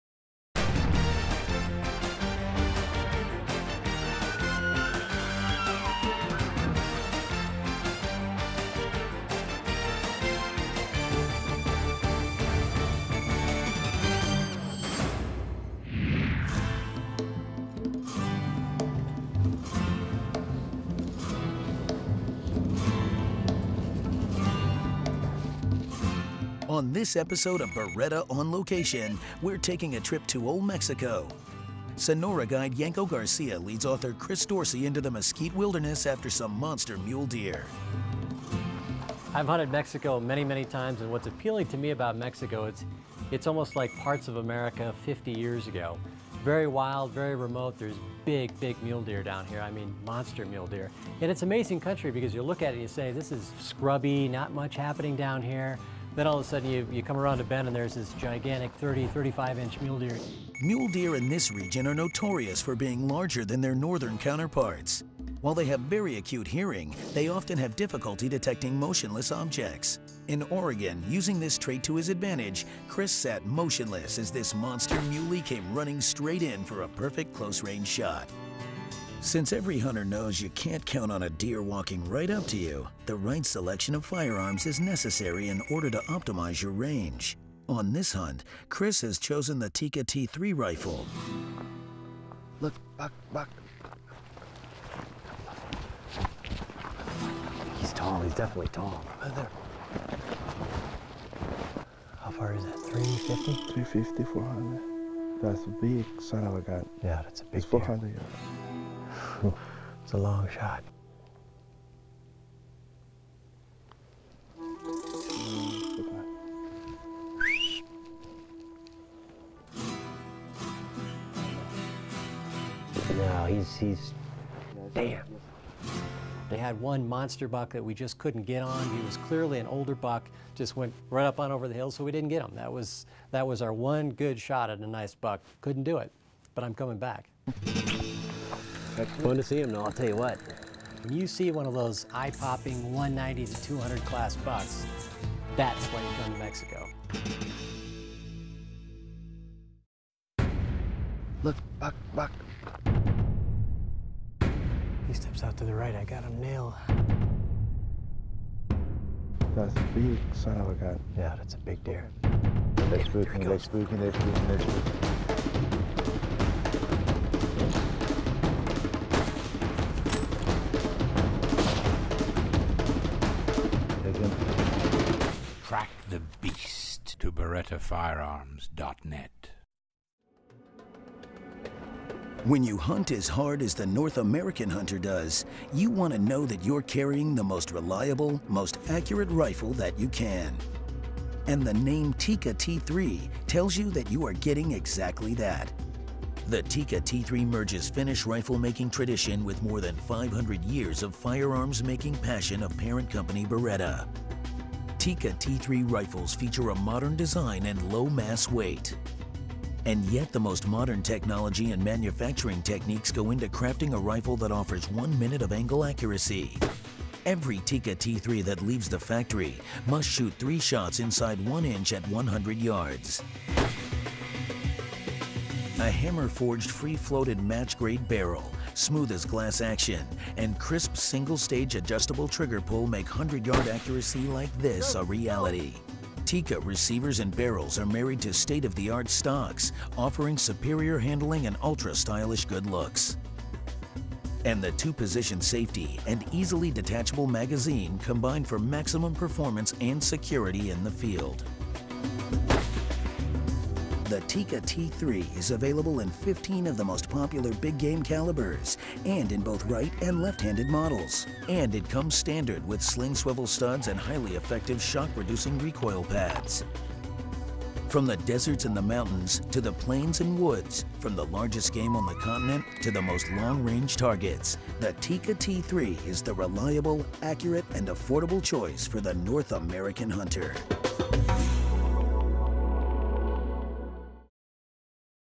Hunting in Mexico with Beretta
Any rifle can boast technology but in the end it all comes down to accuracy, and Sako rifles enjoy the reputation throughout the world as producing the most accurate production rifles on the planet. Berettas new Sako A7 represents the beginning of a new era for the American hunter, joining Sakos legendary Finnish tradition of accuracy and reliability with todays American hunter. With better out-of-the-box accuracy and reliability than any other rifle brand on the market, the new Sako A7 boasts match-grade performance with a price never before seen on a match-grade hunting rifle.